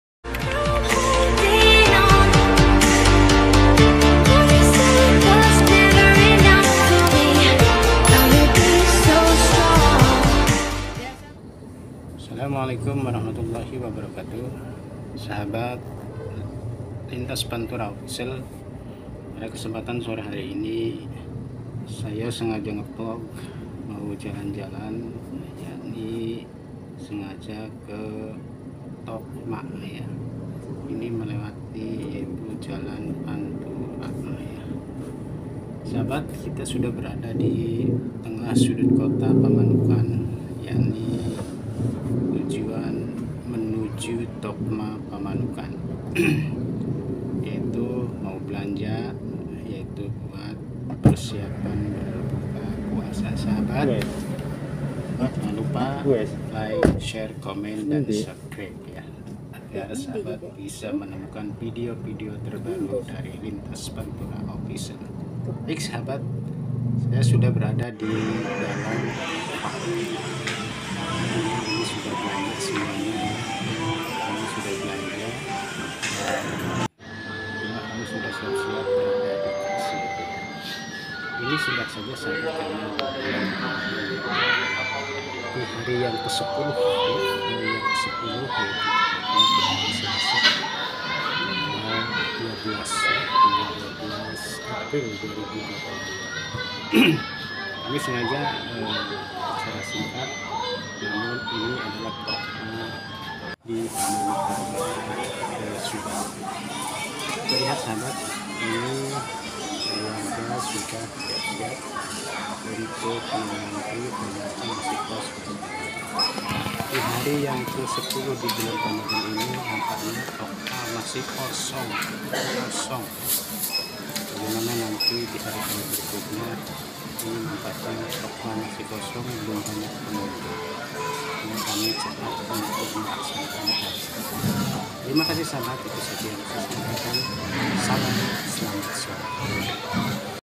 Mengunjungi Tokma Pamanukan di hari ke 10 di bulan Ramadhan 2022
Animo masyarakat untuk berkunjung ke tempat belanja seperti Tokma Pamanukan, di hari ke 10 bulan puasa 2022 masih belum nampak ramai, kenapa ?
yuk cari tahu